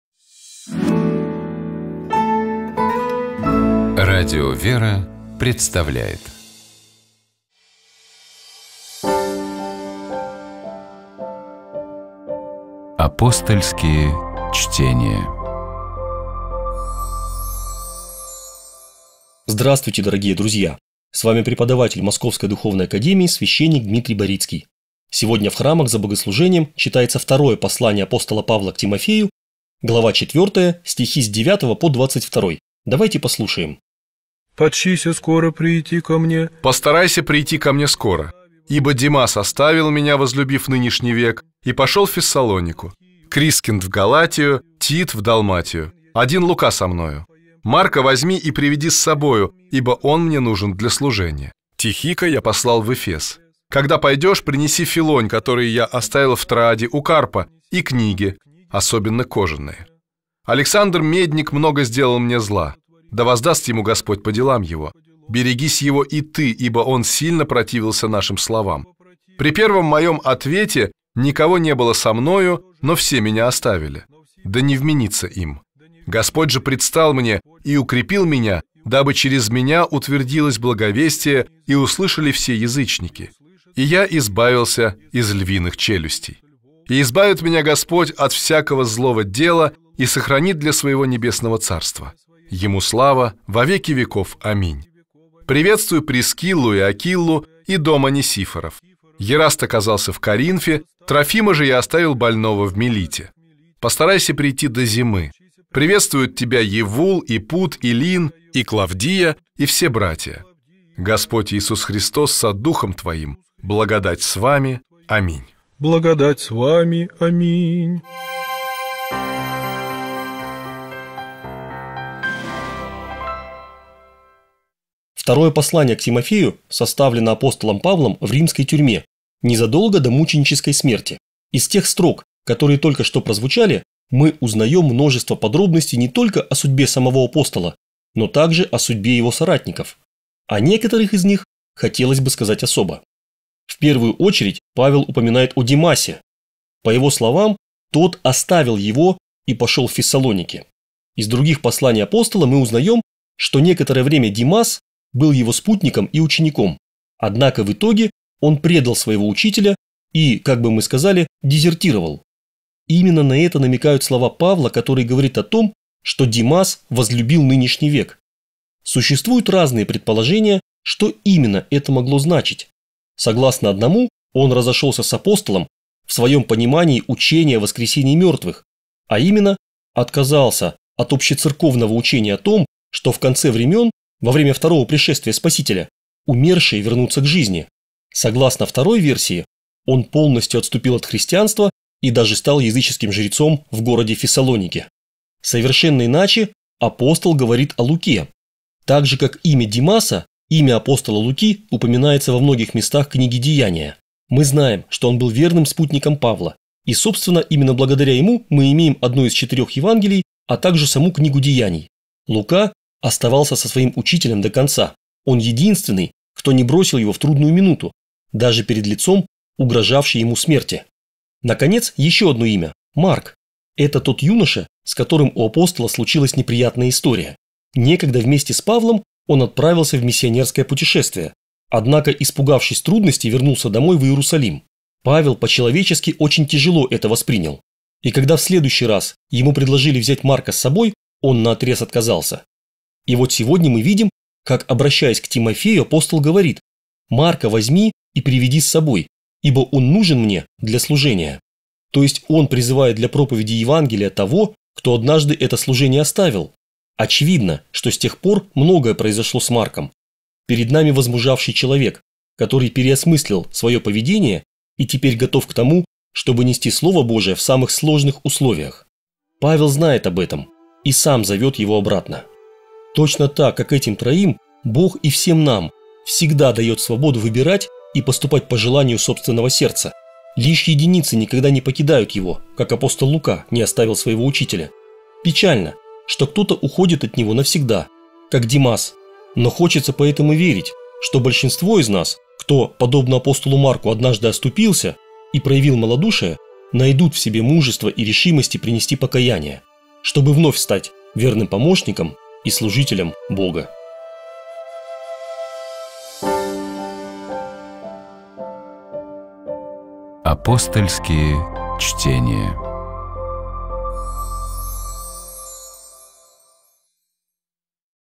Апостольские чтения. Апостол Павел перед Кесарем (2 Тим. IV: 9-22)
Комментирует священник Дмитрий Барицкий